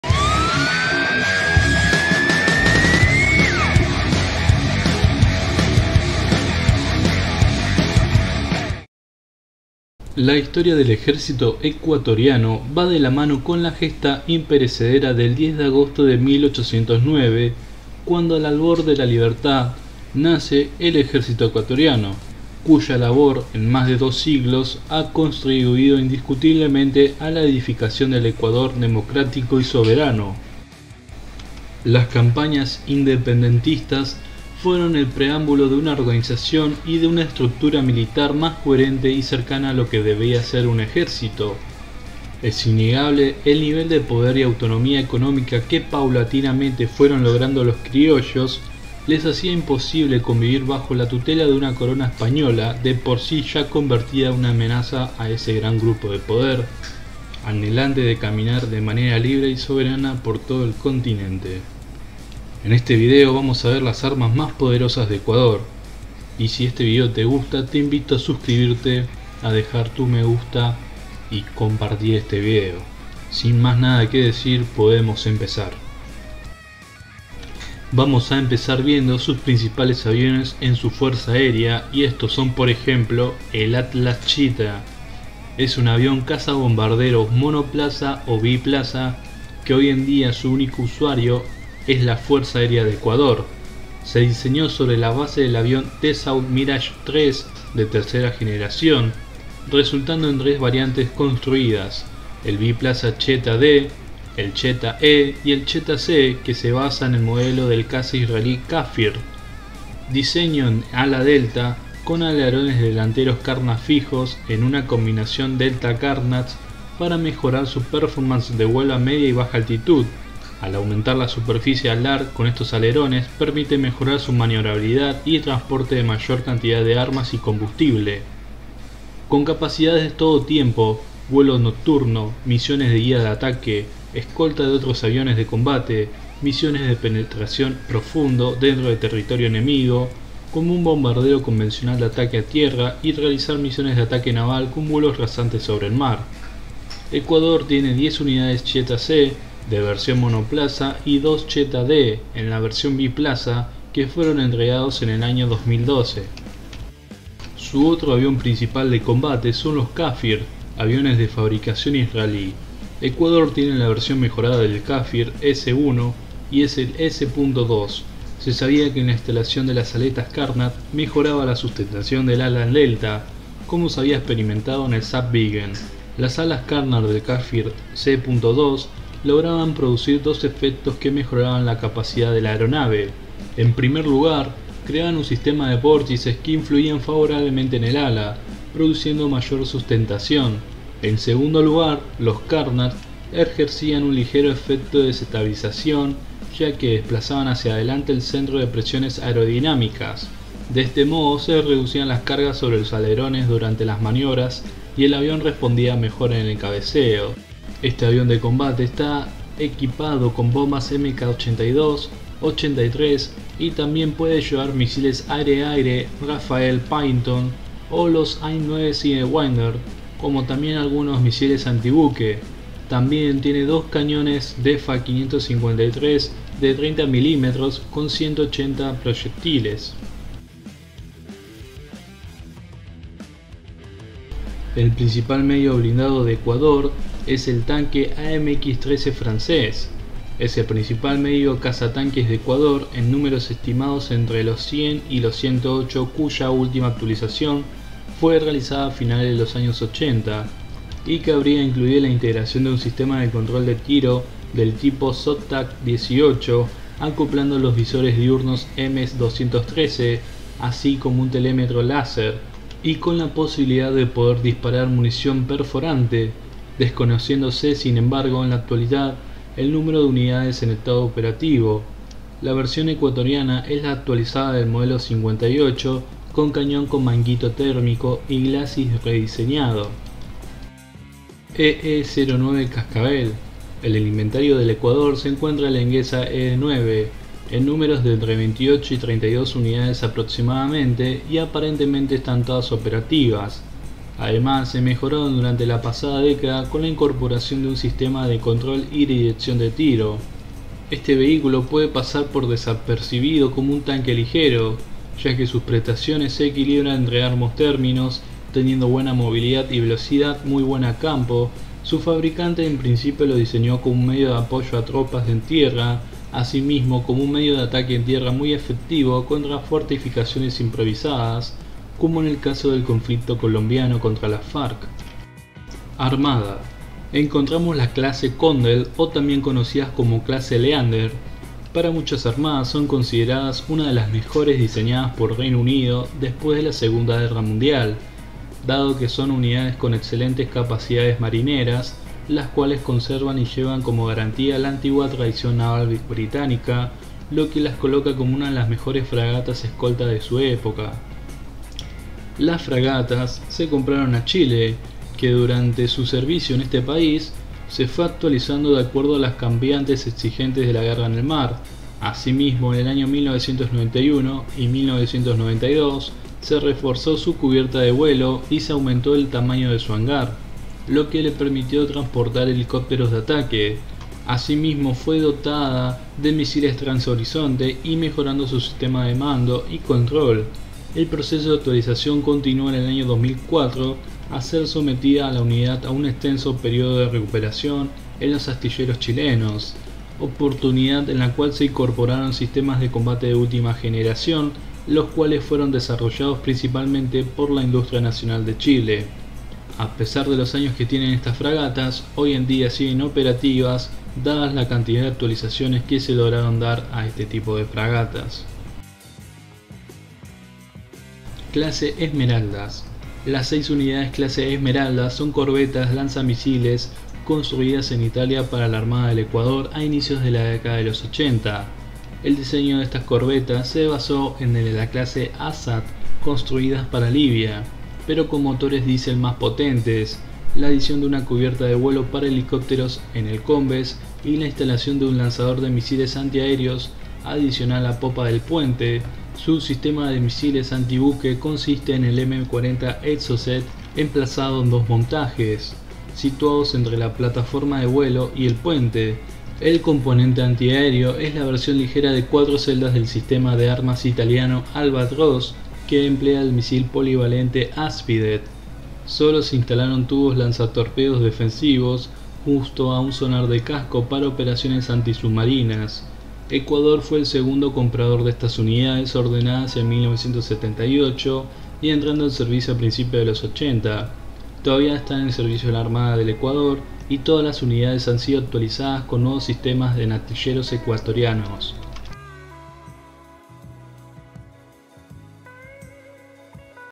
ECUADOR: Armas MAS PODEROSAS Del Ejercito ECUATORIANO 🇪🇨
La historia del Ejército ecuatoriano va de la mano con la gesta imperecedera del 10 de agosto de 1809, cuando al albor de la libertad, nace el Ejército ecuatoriano, cuya labor en más de dos siglos ha contribuido indiscutiblemente a la edificación del Ecuador democrático y soberano.